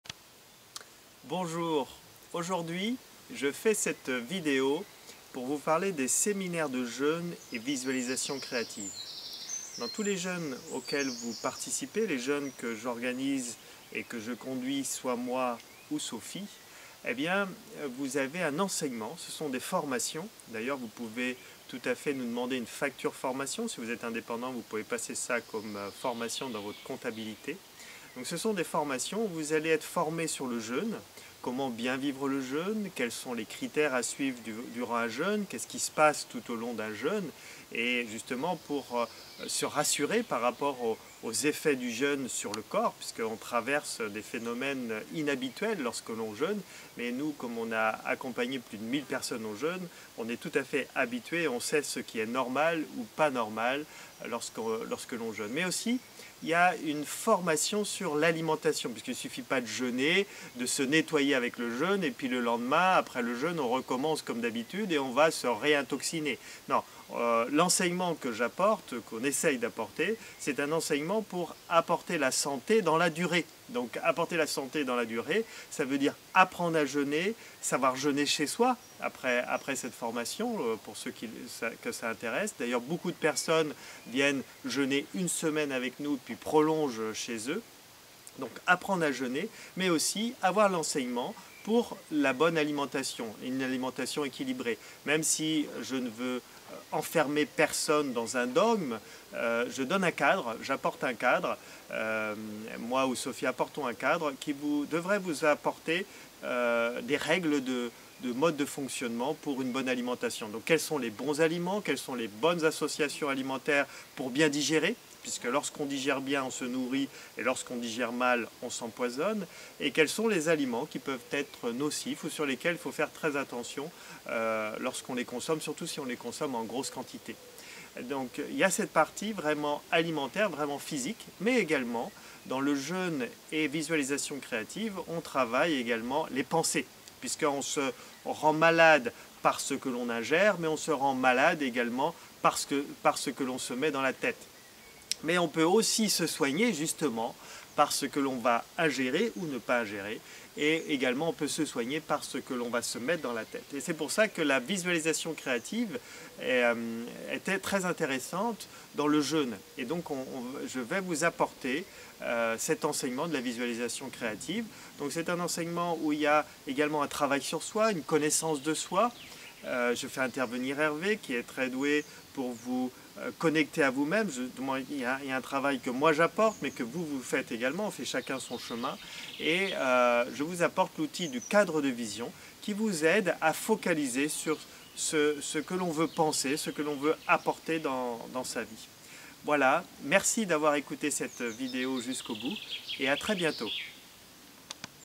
jeûne thérapeutique et visualisation créative
Nous sommes malade de ce que nous absorbons par la bouche et par la tête. Retrouvez la santé en changeant ce que l'on se met dans la bouche et ce que l'on se met dans la tête.
Formation au jeûne et à la pensée juste.